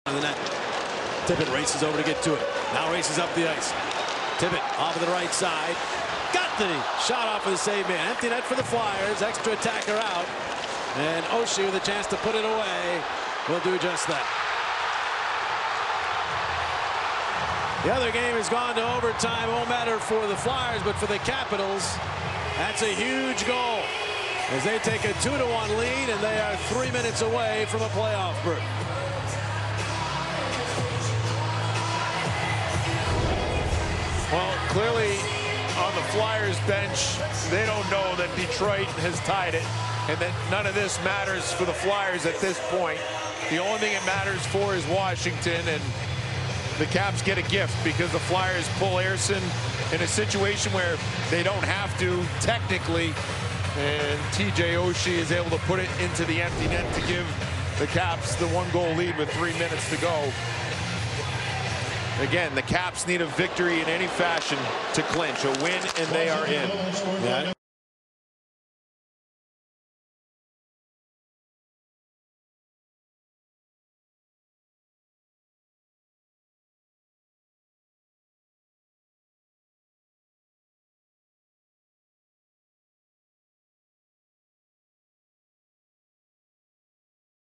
BIZARRE MISUNDERSTANDING LEADS TO WASHINGTON CLINCHING FINAL PLAYOFF SPOT
The Flyers needed a regulation win to make the playoffs so Tortorella pulled the goalie late in a 1-1 game and the Capitals scored immediately, eliminating the Wings, Pens, and Flyers from playoff contention.